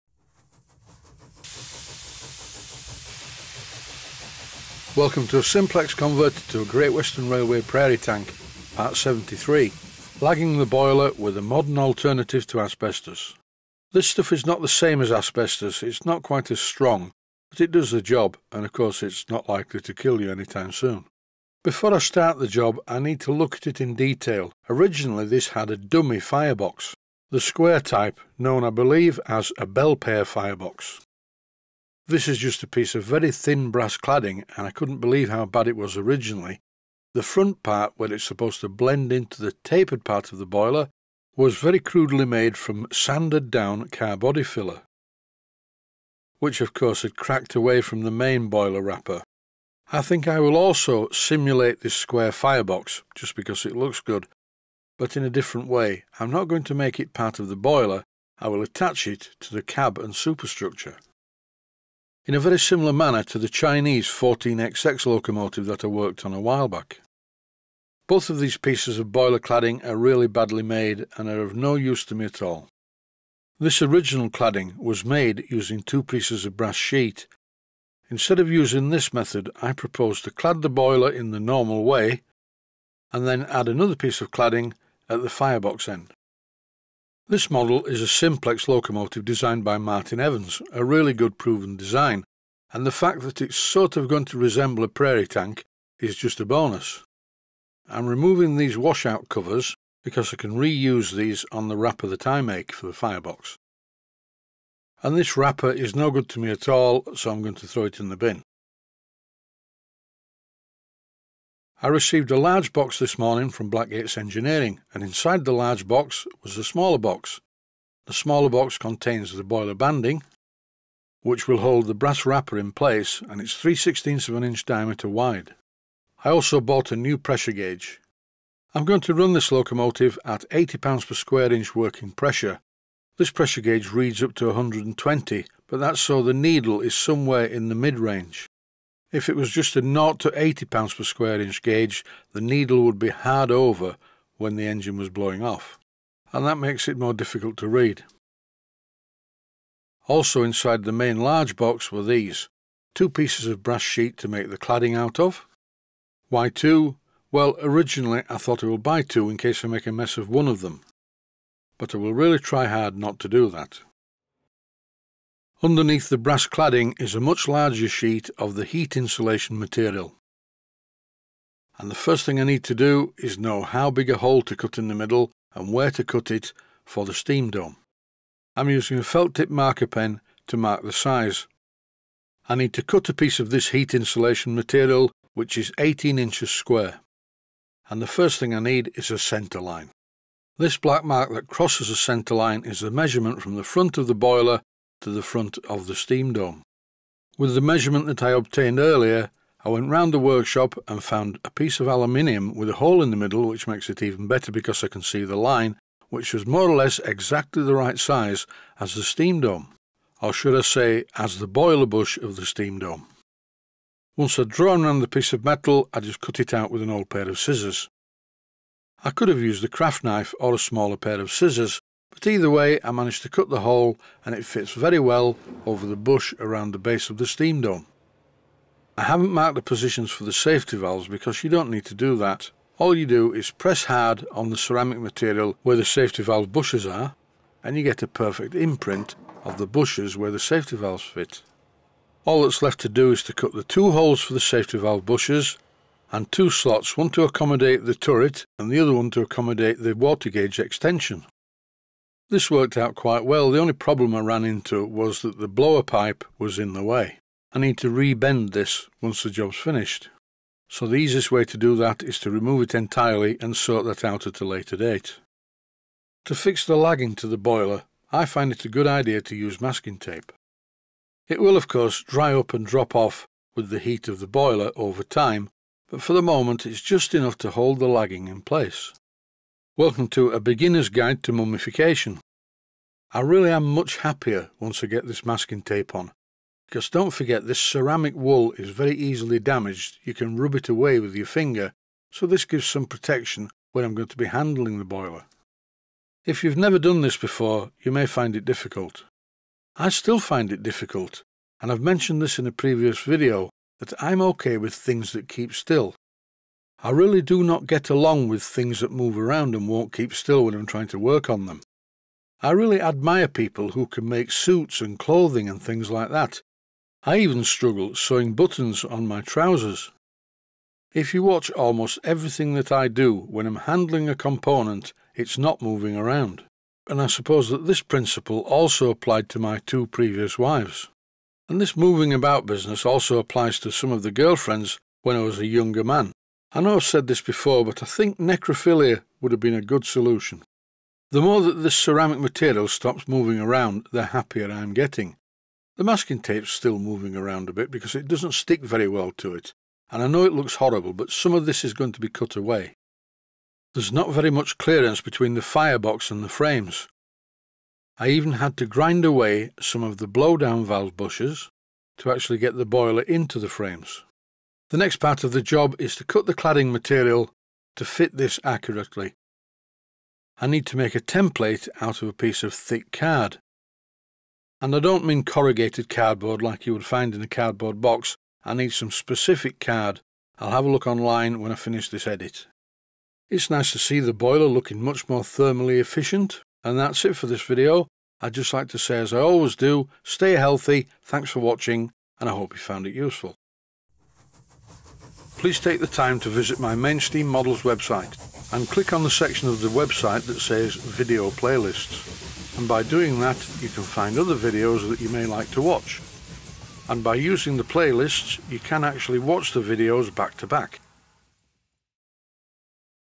SIMPLEX PRAIRIE TANK - PART #73
Simplex Prairie Tank - Part #73. Lagging the Boiler with a suitable modern heat insulation material.